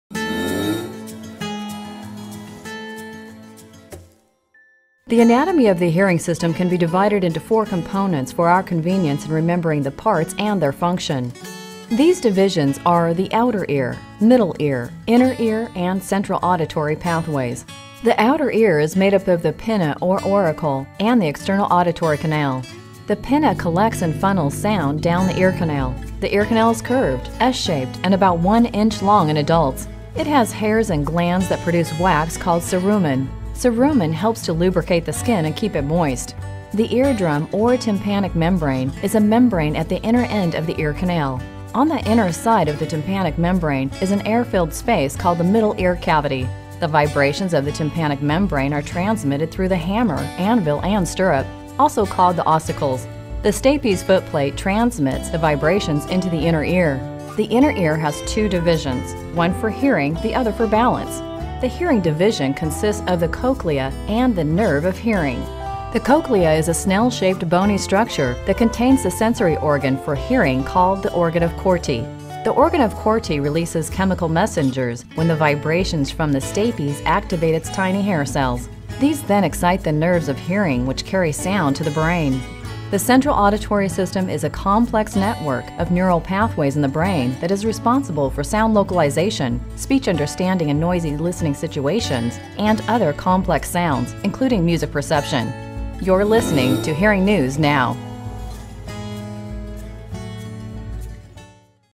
Hearing Explained
How does hearing work? It's actually a complicated yet fascinating process, and this quick video can show you.